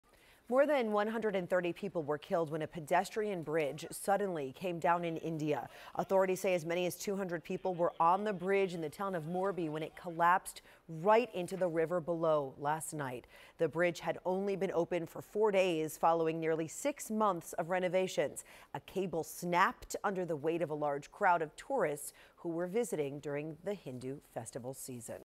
India: More than 130 dead, many injured after bridge carrying hundreds collapses into river
Military teams were hunting Monday for people missing after a century-old cable suspension bridge collapsed into a river Sunday in the western Indian state of Gujarat, sending hundreds plunging into the water and killing at least 132 in one of the worst accidents in the country in the past decade.

At least 177 survivors were pulled from the river and teams from the army, navy and air force were searching for others still missing, said Jigar Khunt, an information department official in Gujarat said.